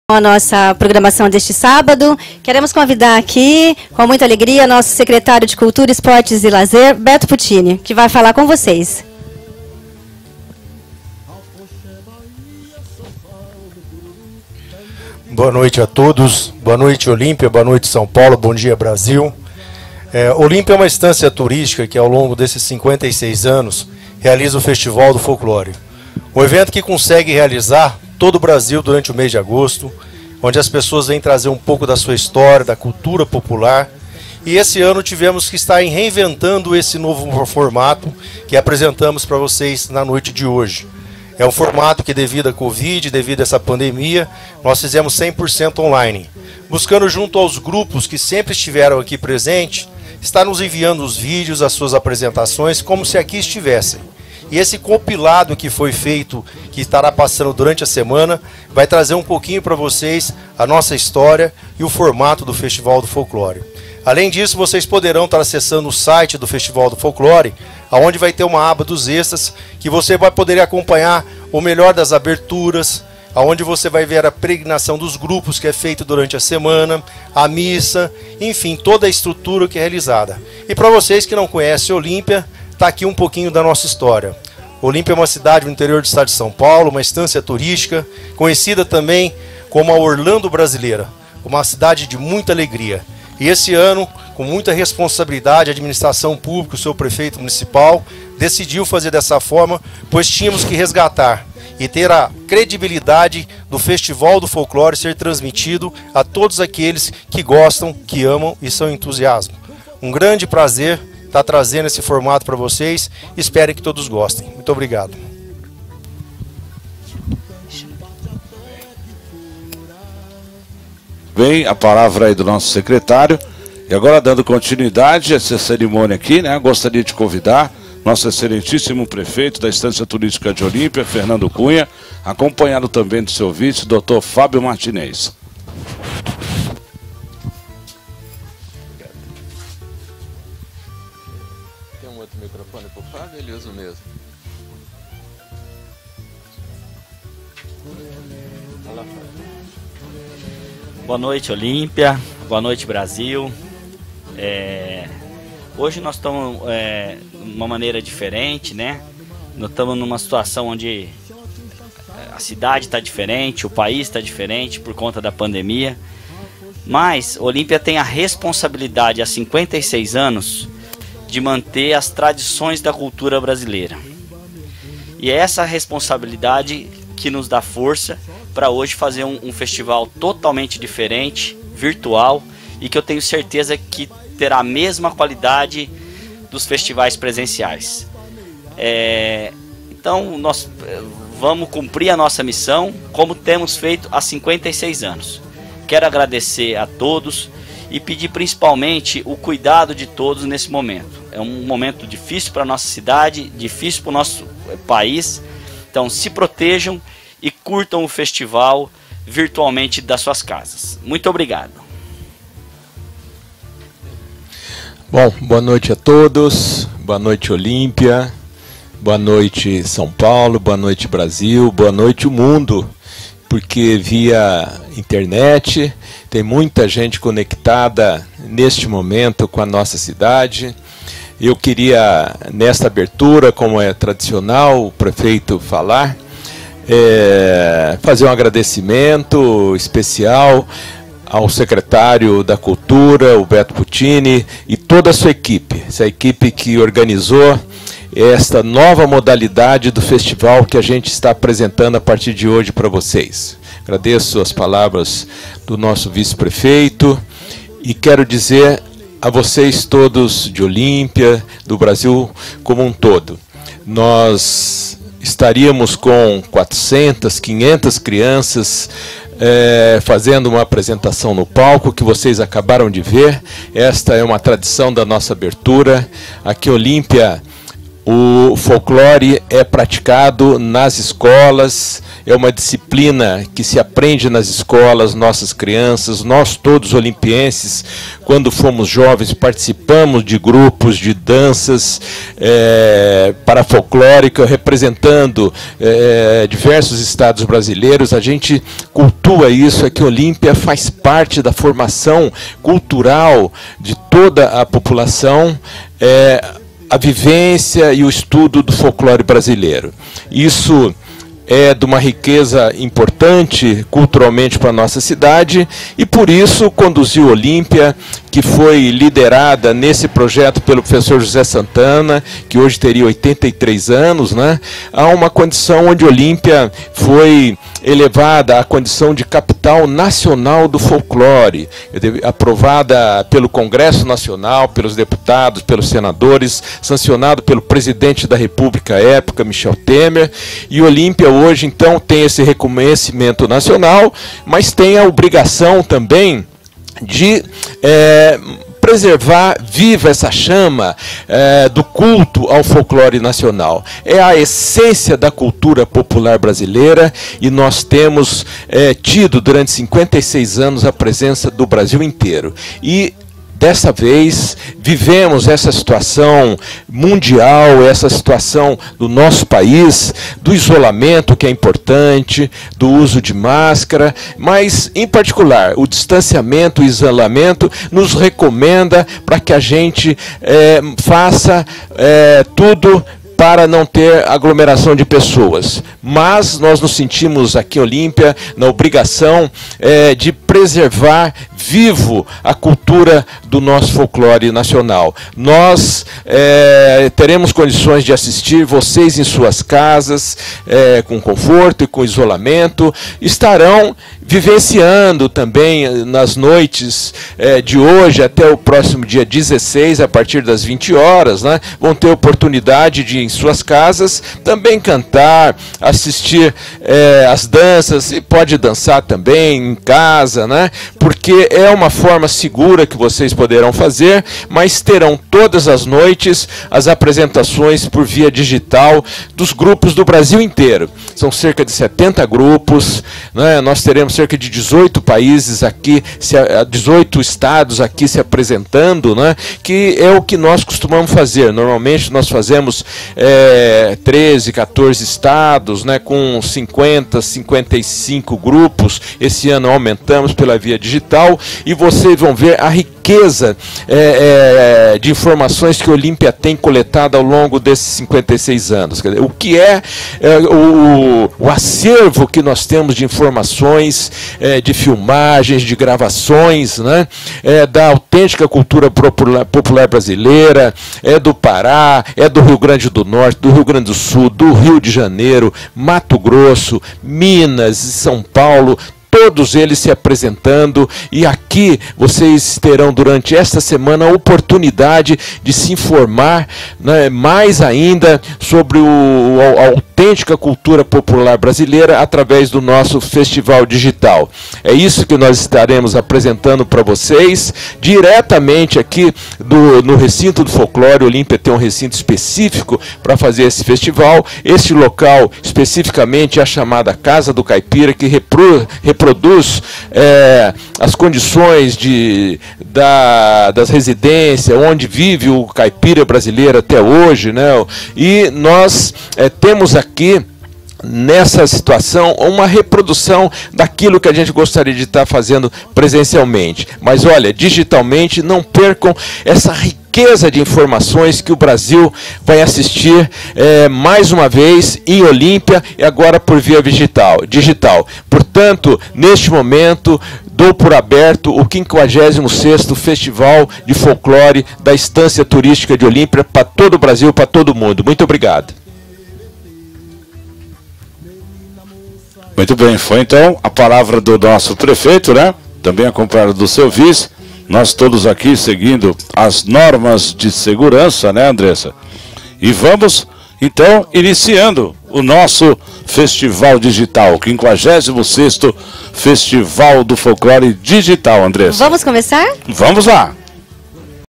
AUTORIDADES ABERTURA FEFOL DIGITAL 2020
Secretário Beto Puttini, Cultura e Turismo; prefeito Fernando Cunha, de Olímpia, e o seu vice, Fábio Martinez, durante a abertura do Festival de Folclore de Olímpia, primeiro digital devido à pandemia.